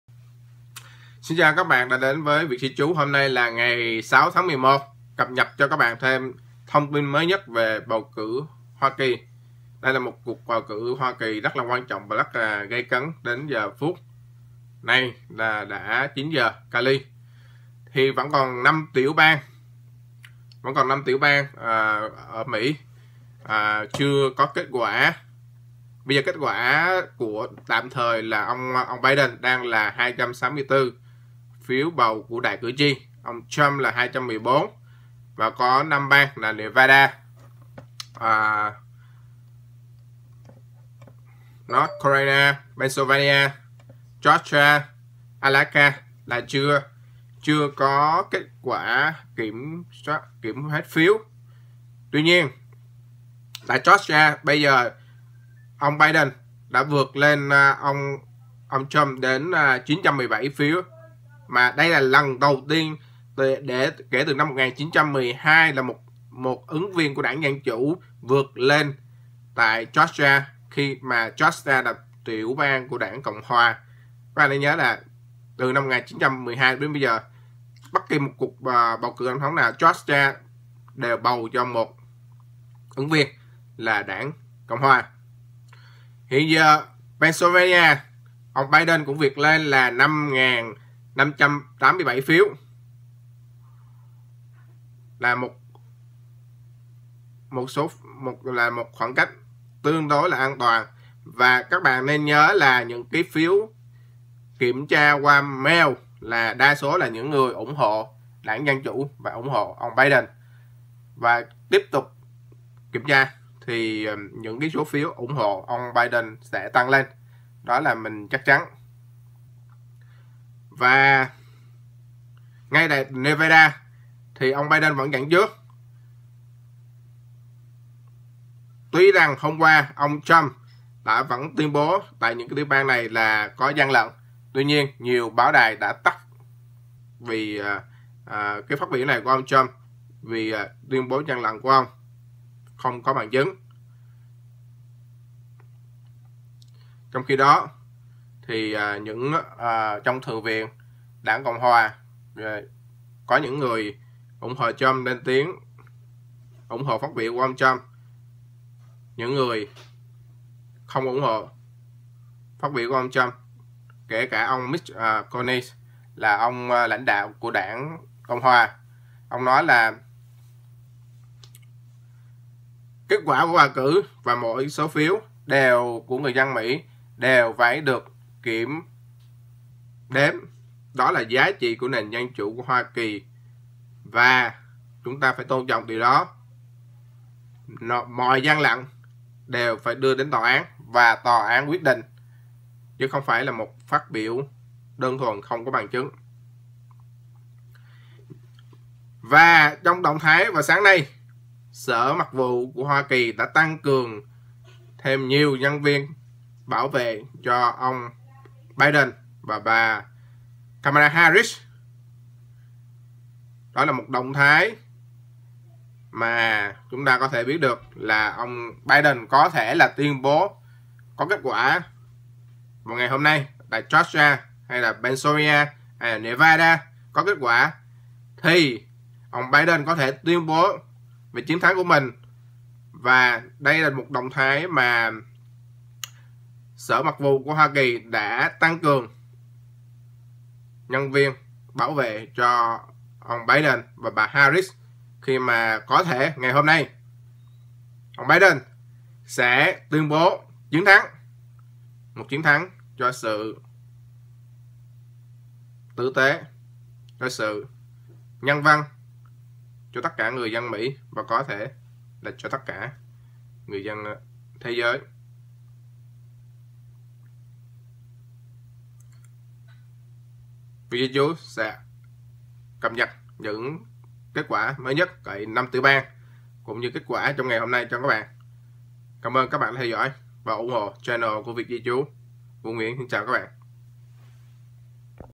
Cập nhật kết quả bầu cử Hoa Kỳ 2020 sáng ngày 11.06.2020 !!!
Ứng viên Joe Biden đã bất ngờ vượt lên dẫn trước đối thủ Donald Trump tại bang chiến trường Pennsylvania, bang nắm giữ 20 phiếu đại cử tri. Kết quả này khiến ông Biden đang rộng cửa đắc cử tổng thống.